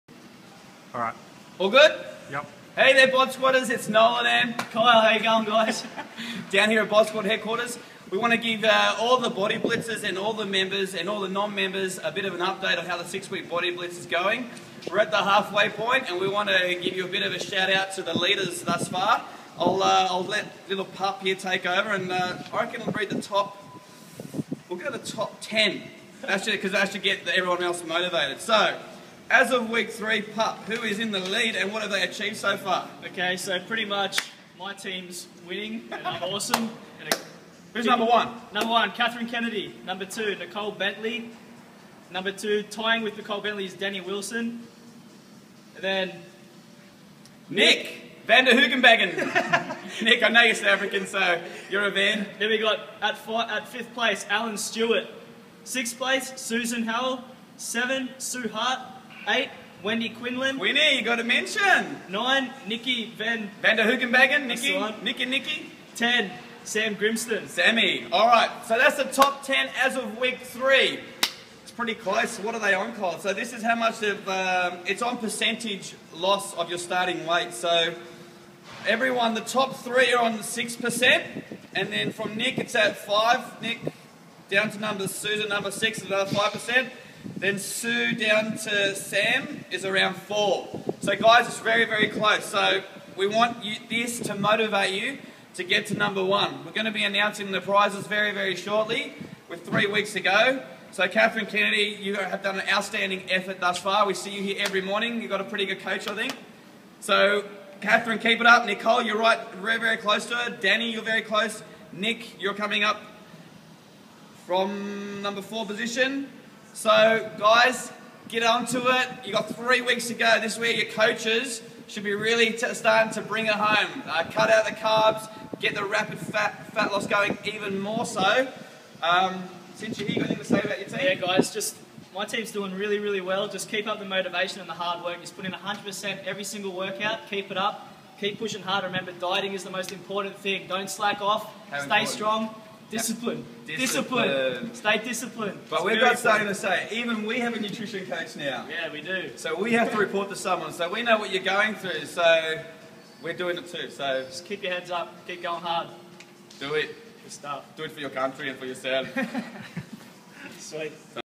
Castle hill boot camp Bodsquad boot camp
The results from week 3 body blitz with Bodsquad castle hill boot camp are in...